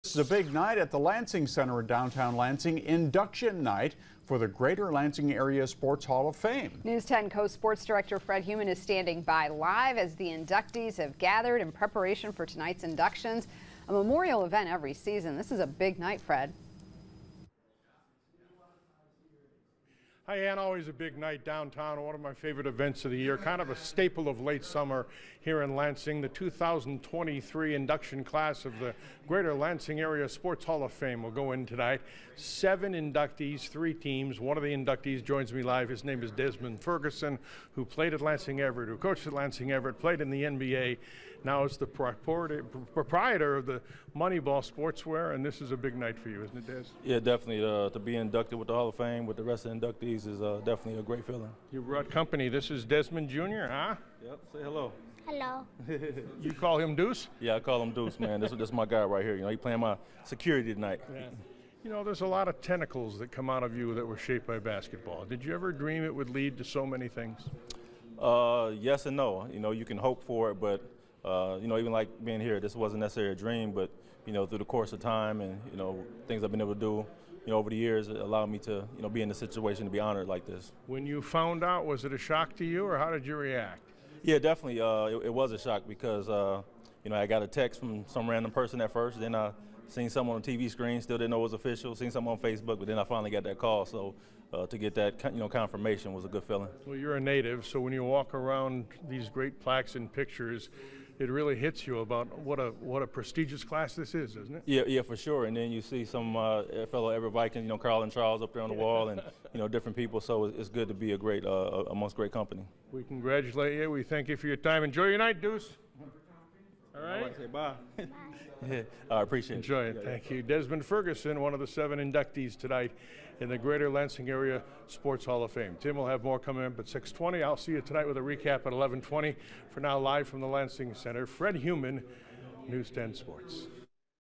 Athletes inducted into Greater Lansing Area Sports Hall of Fame
It's a big night at the Lansing Center.